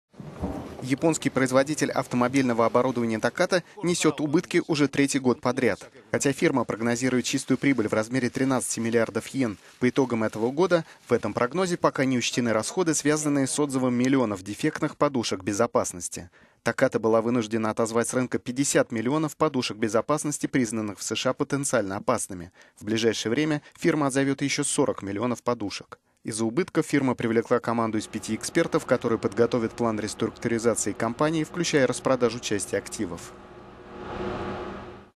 Takata готовится к реструктуризации - corporate
Японский производитель автомобильного оборудования Takata несёт убытки уже третий год подряд. Хотя фирма прогнозирует чистую прибыль в размере 13 млрд йен по итогам этого года, в этом прогнозе пока не учтены расходы, связанные с "отзывом" миллионов дефектных подушек безопасности.

Takata была вынуждена отозвать с рынка 50 млн подушек безопасности, признанных в США потенциально опасными. В ближайшее время…